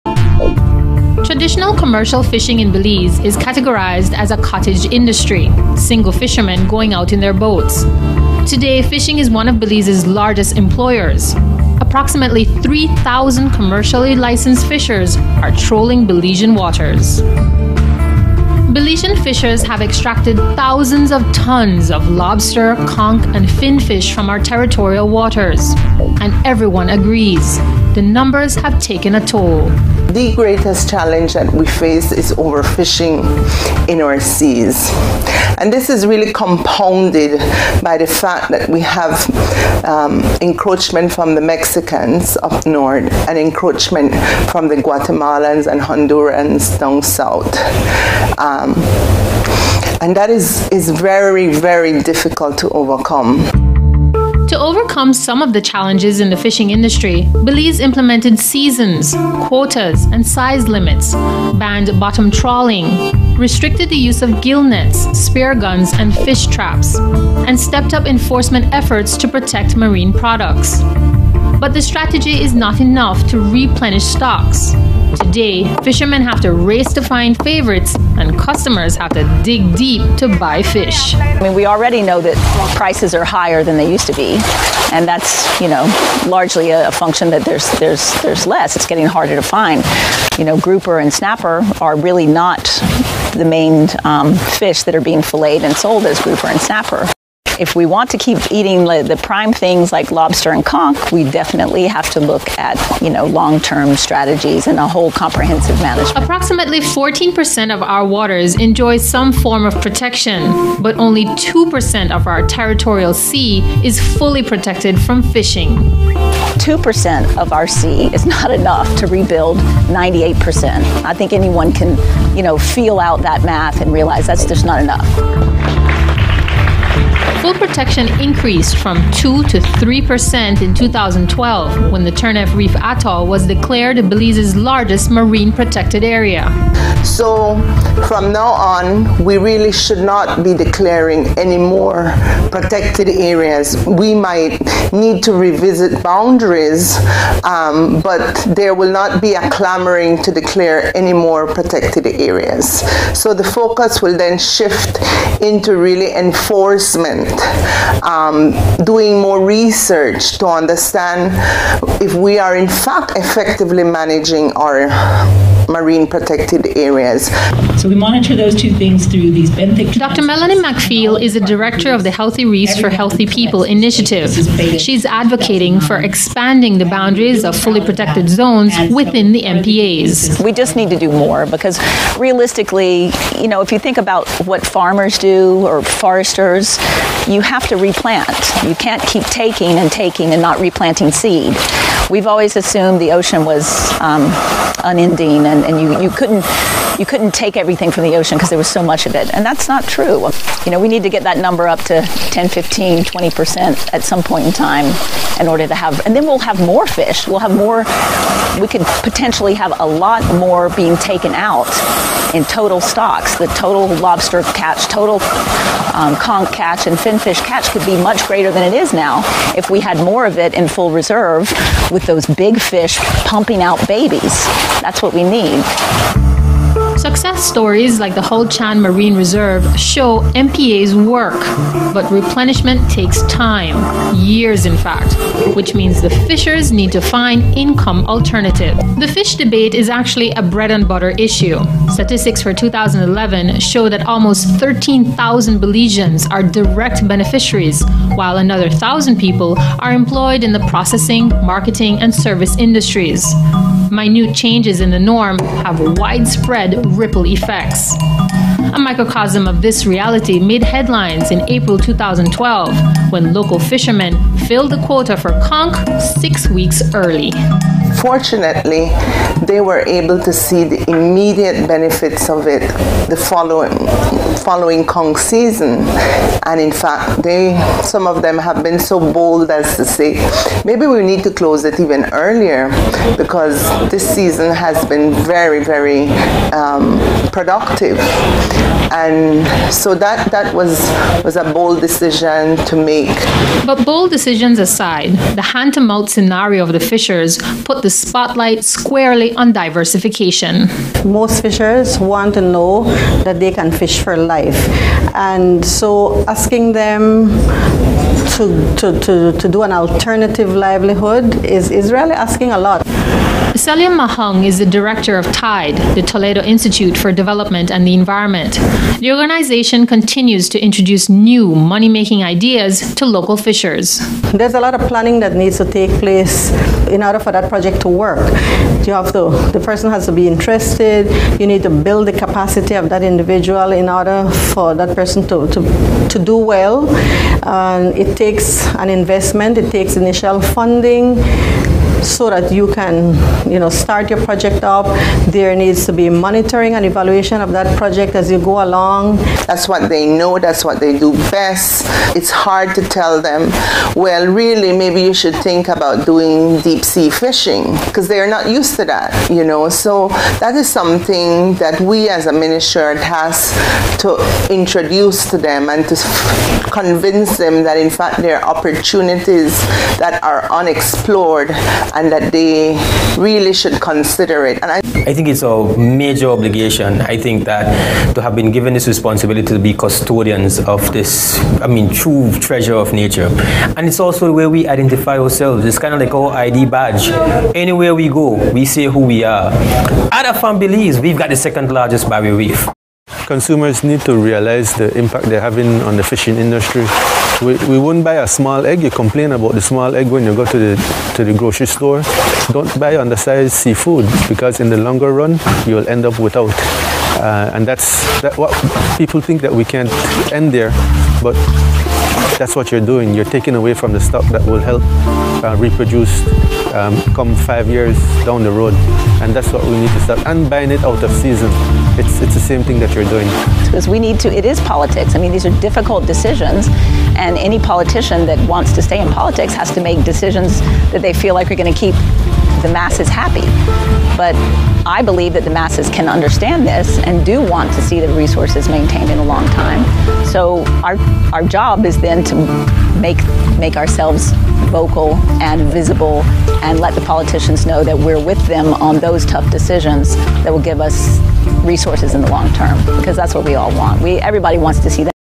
Belize running outta Fish
A Belizean once said "There are many Fish in the sea" Not any more, and certainly not in Belize's Coastal waters... The Mexicans in the North are rippin us off,the Guats. in the south are rippin us off by the hundreds of tons... Even Honduras is rippin us off,every day... And our Coast Guard are acting impotent... How comes the Belizean Coast Guards do such a great job Harassing local fishermen ??? by Mellowvision